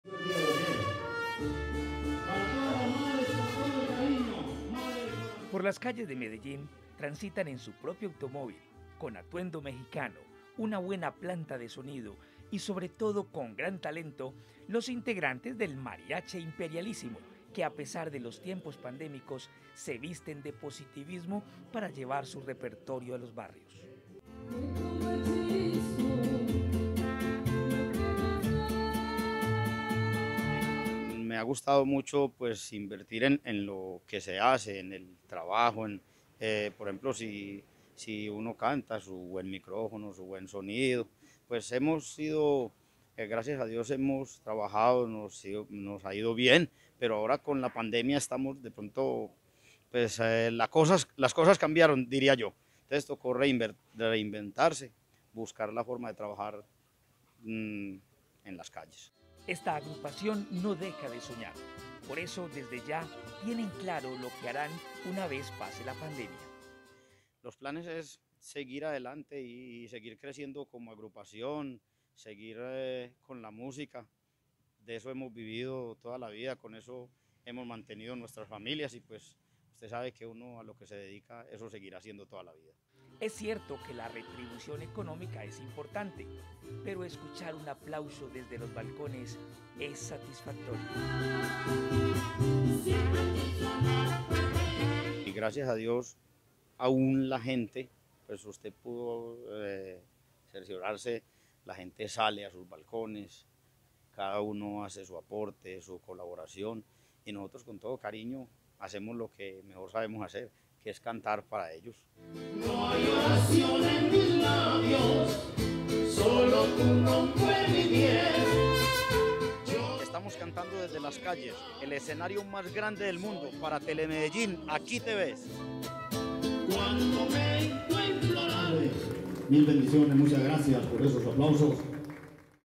Mariachi Imperialismo sigue llevando la música a las calles de Medellín - Telemedellín
NoticiasTM Por las calles de Medellín, transitan en su propio automóvil, con atuendo mexicano, una buena planta de sonido y sobre todo con gran talento; los integrantes del mariachi Imperialísimo, que a pesar de los tiempos pandémicos se visten de positivismo, para llevar su repertorio a los barrios.